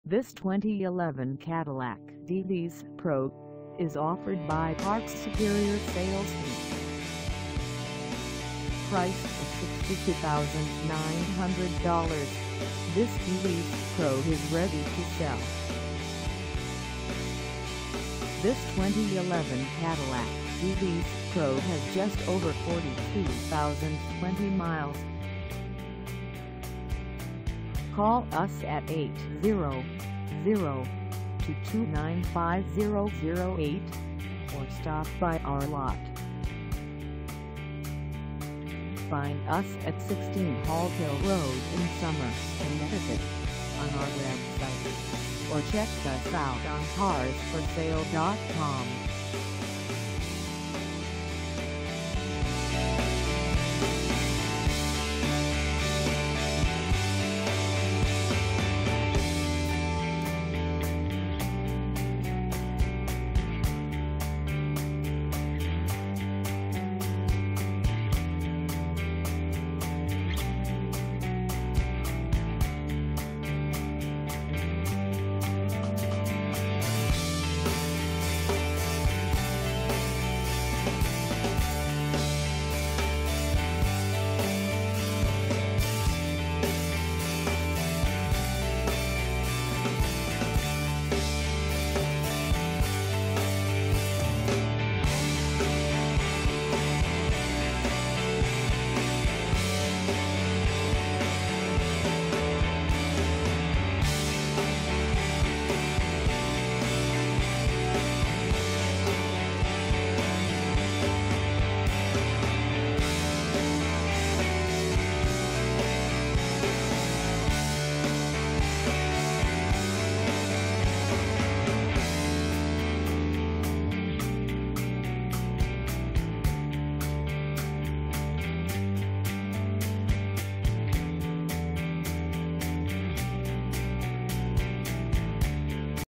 2011 Cadillac DTS Pro Funeral Coach 4dr Sedan for sale in So
This 2011 Cadillac DTS Pro Funeral Coach 4dr Sedan is for sale in Somers, CT 06071 at Parks Superior Sales Inc.

Engine: 4.6L V8
Transmission: Automatic 4-Speed
Color: Black
Mileage: 42020
Contact Parks Superior Sales Inc 16 Hall Hill Road Somers, CT 06071 to test drive this 2011 Cadillac DTS Pro Funeral Coach 4dr Sedan today.